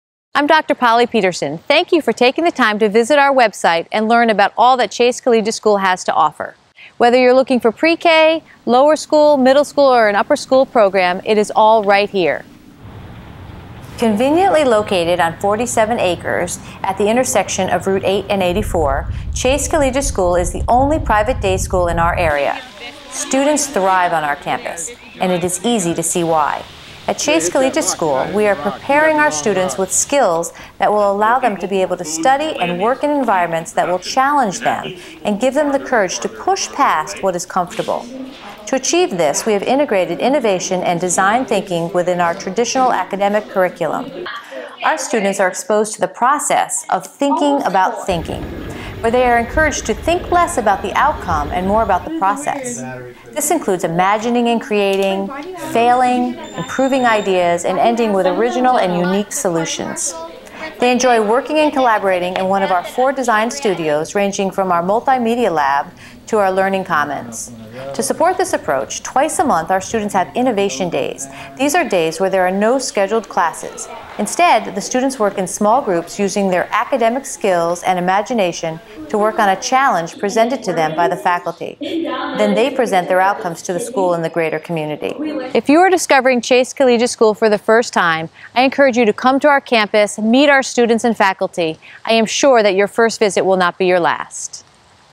Welcome to Chase Collegiate School
Dr. Polly Peterson, Head of School, introduces Chase Collegiate School - a preK through grade 12 independent day school located on 47 acres in Waterbury, CT